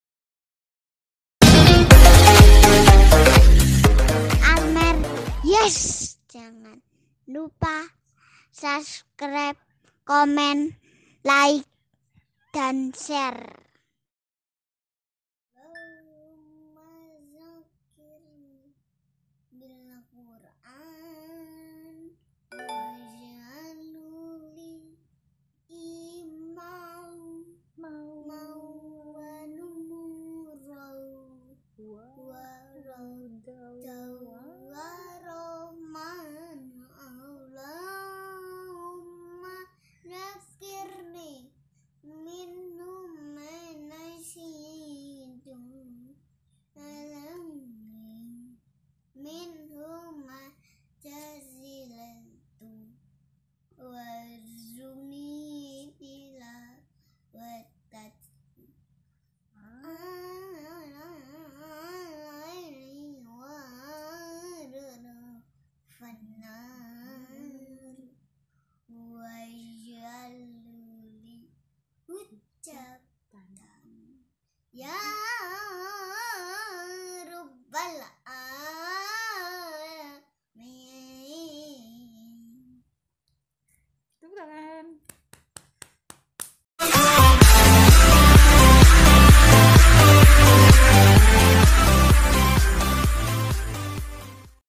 Do'a Khatam Al-quran
Do'a khatam Al-quran ternyata Almer sudah hapal